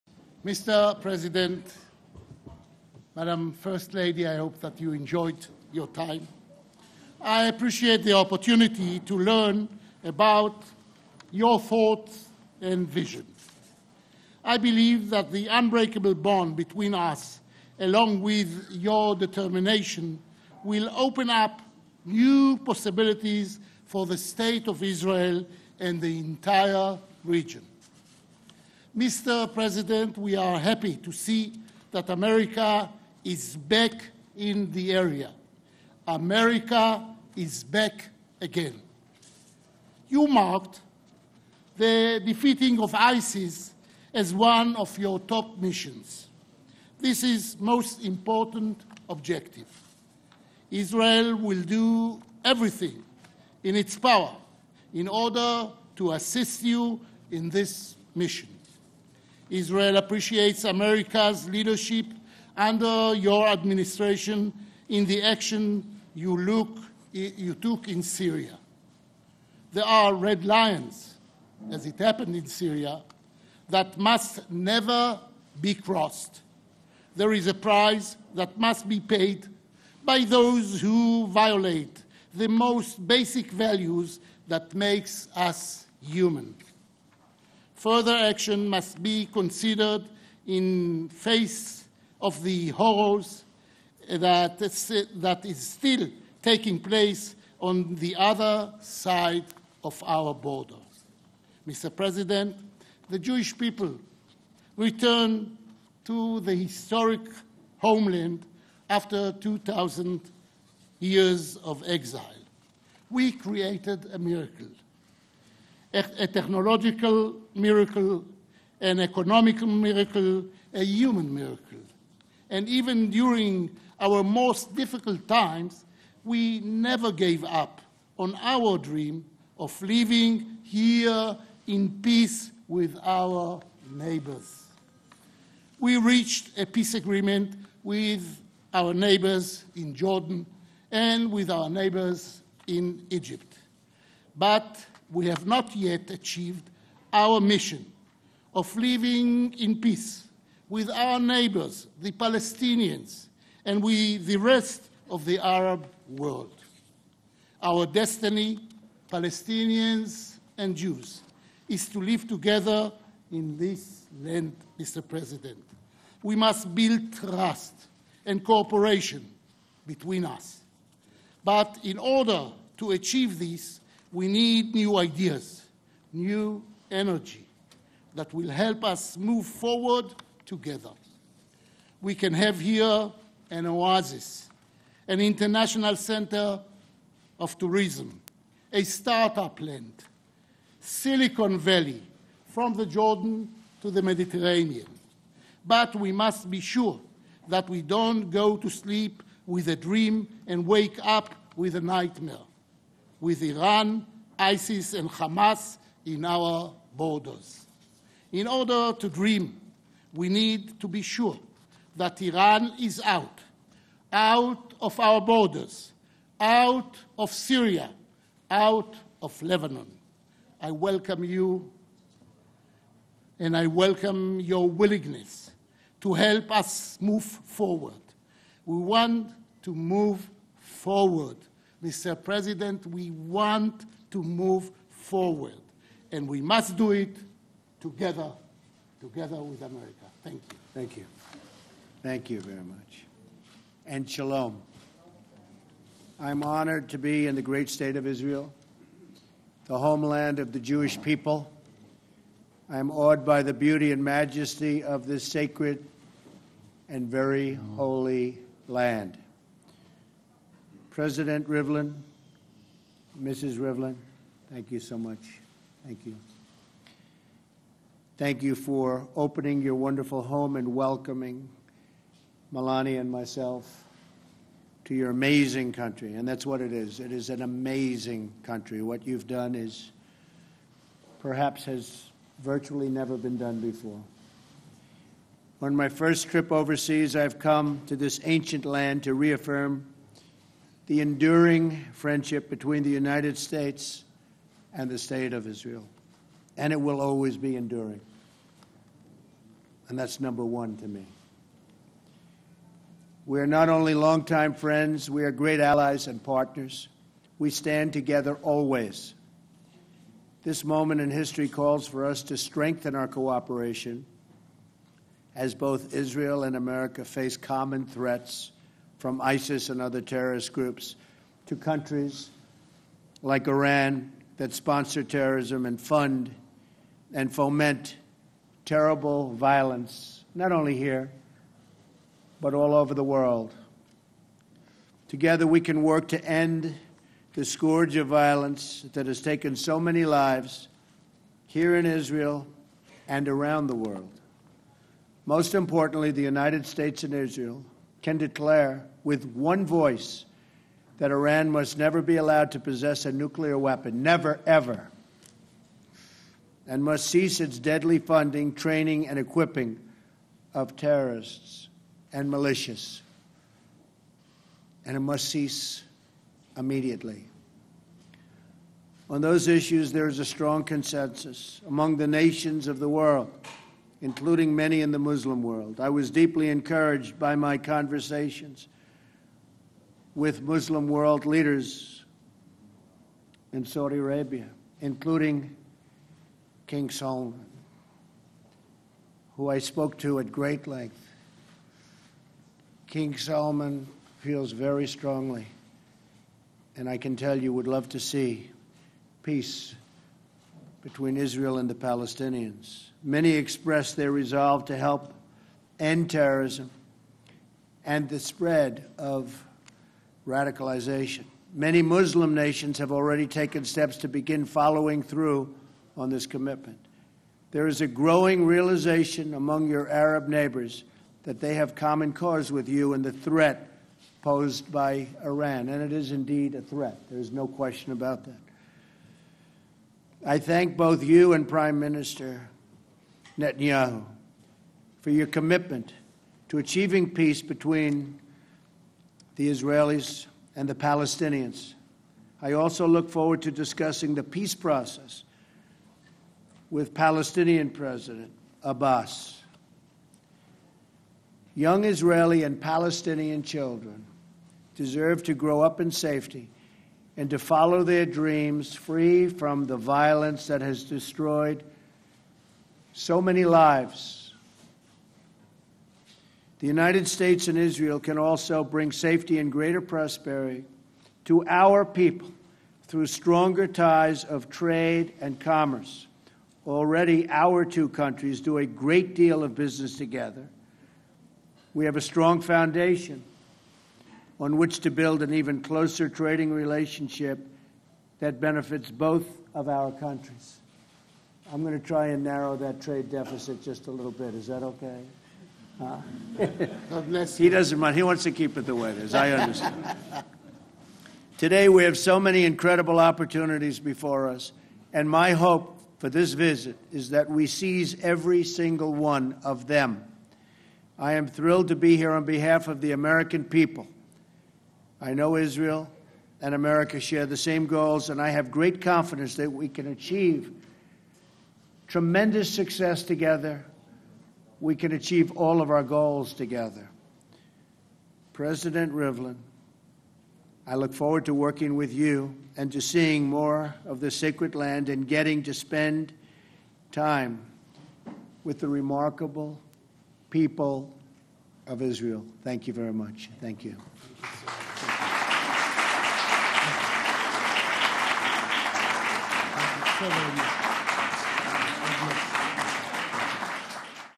Trump and Rivlin deliver joint remarks
President Trump and Israeli President Reuven Rivlin delivered joint remarks before the press in Israel on May 22.